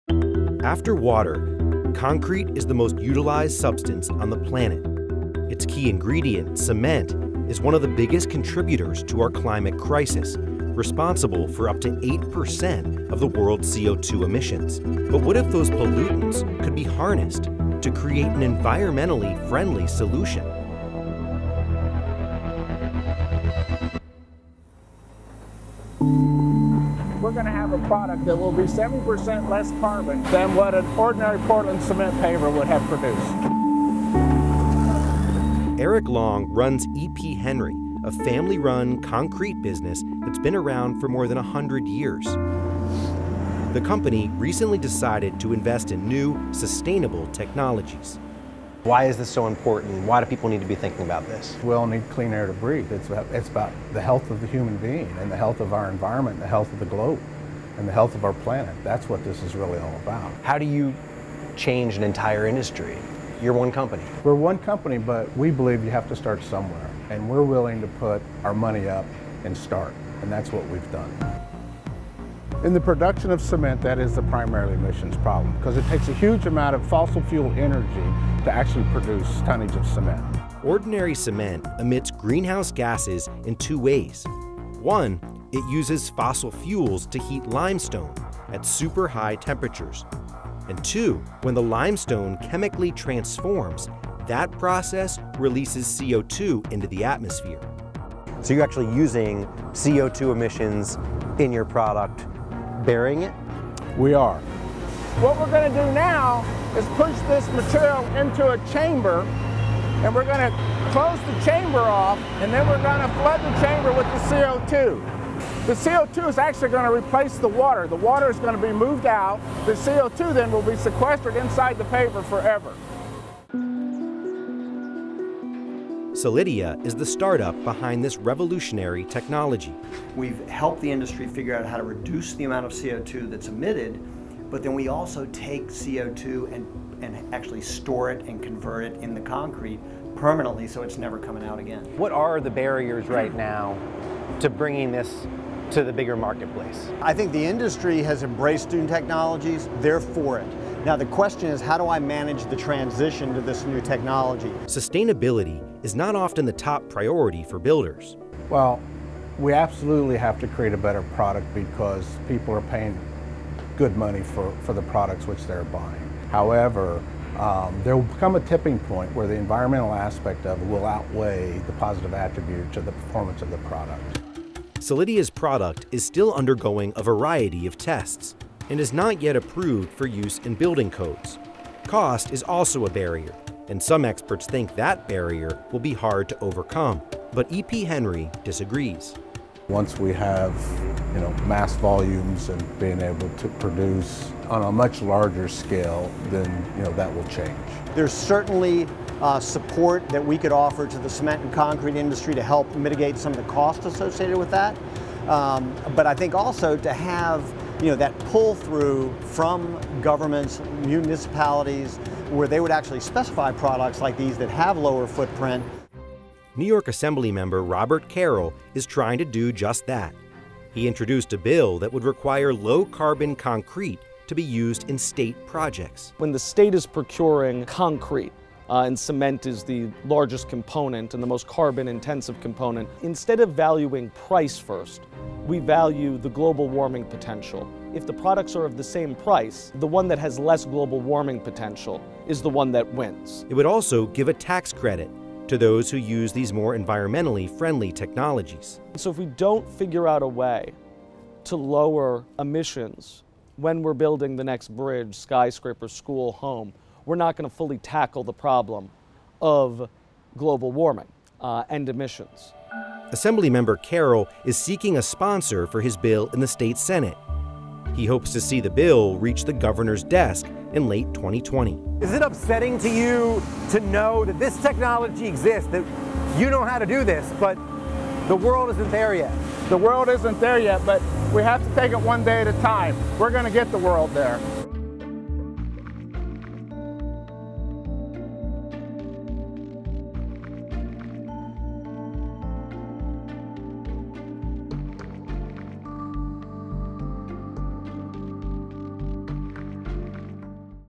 Here's How the Cement Industry Can Lower its Carbon Footprint | NowThis
Cement is responsible for up to 8% of the world’s CO2 emissions — but what if those pollutants could be recycled instead of released?

In US news and current events today, concrete is the most utilized substance on the planet after water. Its key ingredient—cement—is one of the biggest contributors to the climate crisis; cement is responsible for nearly 10% of the world's carbon dioxide emissions, a greenhouse gas. So how exactly can the world help make cement and concrete more sustainable?

Enter: Eric Long. Eric Long operates a family-run concrete business that's been around for more than a century. And the company recently decided to invest in new, sustainable technologies to help the cement industry lower its carbon footprint and, ultimately, help mitigate climate change and fix the environment.